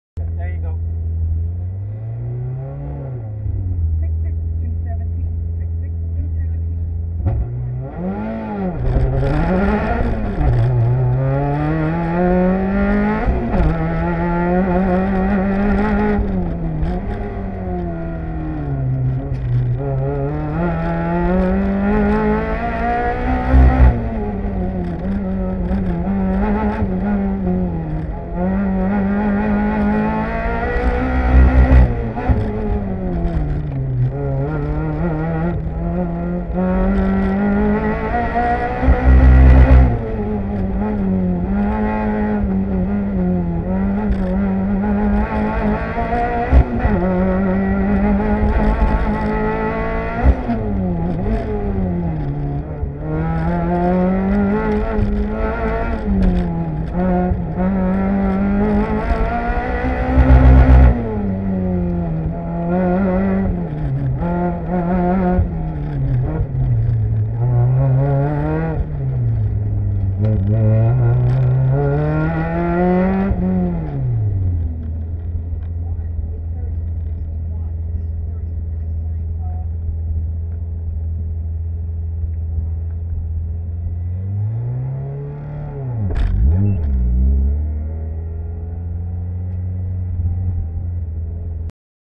The Bunker Monkey racing the Miata
Karl takes my CSP Miata out for an "X" run on Nov. 4th, 2007 @ Fontana, CA.  Taken with a ChaseCam PDR100 recorder, 2 x ChaseCams, and an SS1000 Video processor.